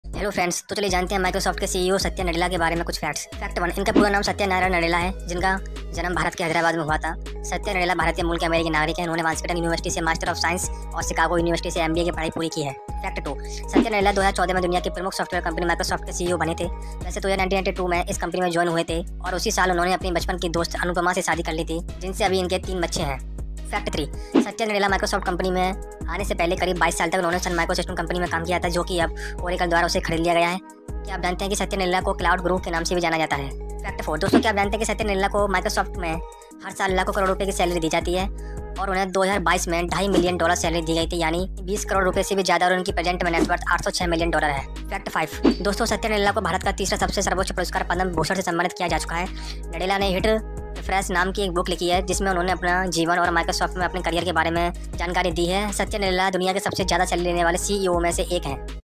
"5 Things You Didn't Know About Satya Nadella"
Facts about Microsoft CEO Satya Nadella

F1
His full name is Satya Narayan Nadella, who was born in Hyderabad, India. Satya Nadella is an American citizen of Indian origin. He has completed Master of Science from Washington University and MBA from University of Chicago.
F2
Satya Nadella became the CEO of the world's leading software company Microsoft in 2014, although he joined this company in 1992 and in the same year he married his childhood friend and Anupama, since then they have three children.
F3
Before coming to Satya Nadella Microsoft Company, he worked in Sun Microsystem Company for about 22 years, which has now been bought by Oracle. Do you know that Satya Nadella is also known as Cloud Guru.
F4
Friends, do you know that Microsoft gives salary in lakhs and crores every year to Satya Nadella, he got salary of two and a half million dollars in 2022 i.e. more than 2000000 crores and his present net worth is 806 million-dollars.
F5
Friends, Satya Nadella has been awarded Padma Bhushan, India's third highest award. Nadella has written a book named Hit Refresh, in which there is information about his life and his career in Microsoft. Satya Nadella is the highest paid CEO in the world. are one of

F1
इनका पूरा नाम सत्य नारायण नडेला है जिनका जन्म भारत के हैदराबाद में हुआ था सत्या नडेला भारतीय मूल के अमेरिकी नागरिक हैं  उन्होंने वाशिंगटन यूनिवर्सिटी से मास्टर ऑफ साइंस और शिकागो यूनिवर्सिटी से एमबीए की पढ़ाई पूरी की है
F2
सत्य नडेला 2014 में दुनिया की प्रमुख सॉफ्टवेयर कंपनी माइक्रोसॉफ्ट के सीईओ बने थे वैसे तो यह 1992 में इस कंपनी में ज्वाइन हुए थे और उसी साल उन्होंने अपनी बचपन की दोस्त और अनुपमा  से शादी कर ली थी दिन से अभी इन्हें तीन बच्चे हैं
F3
सत्य नडेला माइक्रोसॉफ्ट कंपनी में आने से पहले करीब 22 साल तक उन्होंने सन माइक्रोसिस्टम कंपनी में काम किया था जो कि अब ओरेकल द्वारा खरीद ली गई है क्या आप जानते हैं की सत्या नडेला को क्लाउड गुरु के नाम से भी जाना जाता है
F4
दोस्तों क्या आप जानते हैं कि सत्य नडेला को माइक्रोसॉफ्ट हर साल लाखों-करोड़ों में सैलरी देती है उन्हें 2022 में ढाई मिलियन डॉलर सैलरी मिली थी यानी 2000000 करोड़ से भी ज्यादा और इनकी प्रजेंट में नेटवर्थ 806 million-dollar है
F5
दोस्तों सत्य नडेला को भारत का तीसरा सर्वोच्च पुरस्कार पद्म भूषण से सम्मानित किया जा चुका है नडेला ने हिट रिफ्रेश नाम की एक बुक लिखी है जिसमें उनका जीवन और माइक्रोसॉफ्ट में उनका करियर के बारे जानकारी है सत्य नडेला दुनिया में सबसे ज्यादा सैलरी लेने वाले सीईओ में से एक हैं